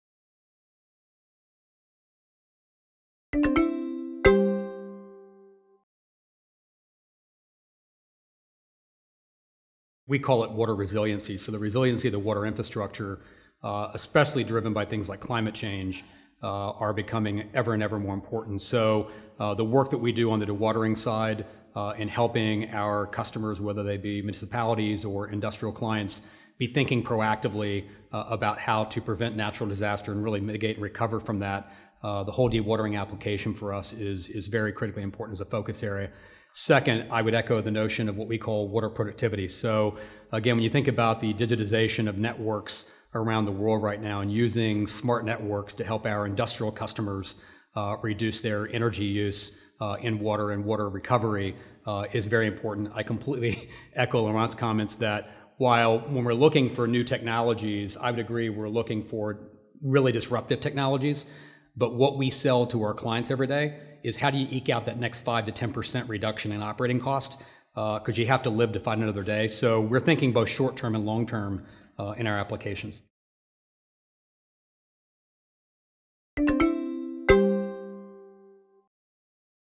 SIWW 2014 TechXchange: Patrick Decker on Solving Water Challenges
Speaking at TechXchange, a one-day forum at Singapore International Water Week (SIWW) 2014, Patrick Decker, president and CEO of Xylem, identifies the water challenges facing the industry.